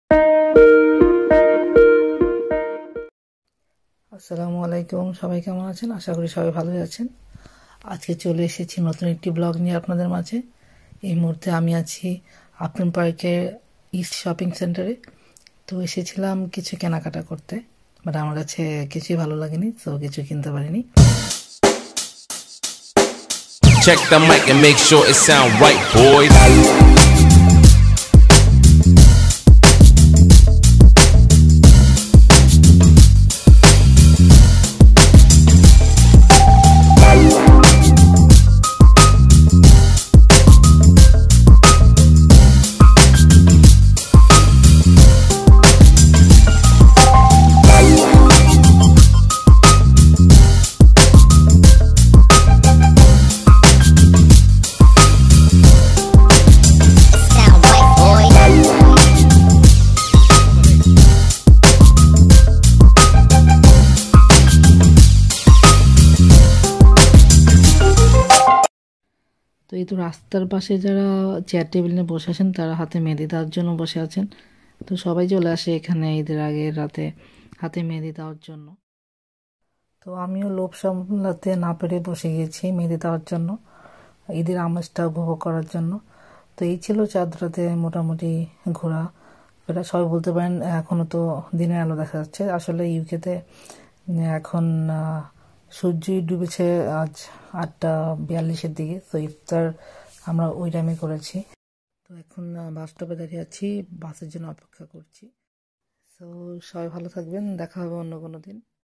Chaad Raat in East London, London, United Kingdom
I have shared a small vlog of the day before Eid (Chaad Raat). East Londoner celebrates chaad raat by sitting at road side of Upton park by painting hena on people's hands.